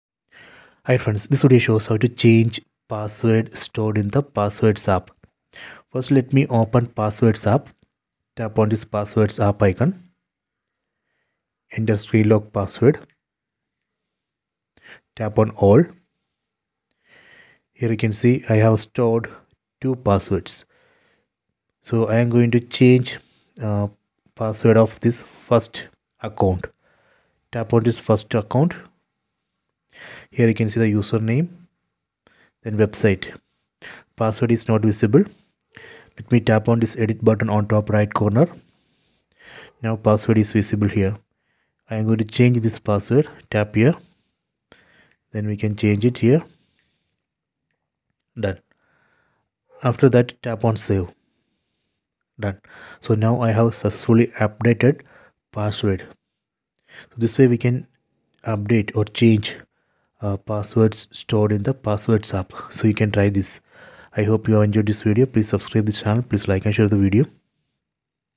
iOS 18 | How to Change Passwords Stored in the Passwords App on iPhone | Easy Guide
In this video, learn how to change passwords stored in the Passwords app on iOS 18. This easy-to-follow guide will show you how to update your credentials securely and efficiently. Whether you're changing passwords for security reasons or simply updating them, this tutorial will help you manage your passwords on iPhone or iPad with ease. Keep your accounts safe and organized by following these simple steps on iOS 18.